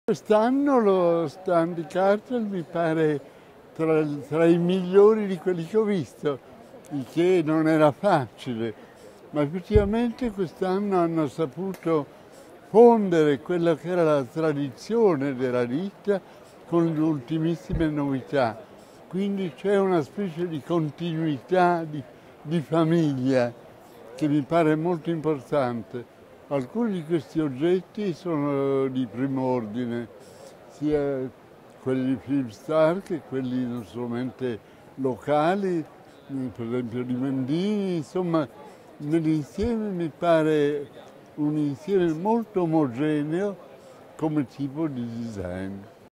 Kartell Contemporary Lifestyle | Interview to Gillo Dorfles
Gillo Dorfles and his point about Kartell Contemporary Lifestyle at Salone 2015.
and sign up to our newsletter to be always updated with our news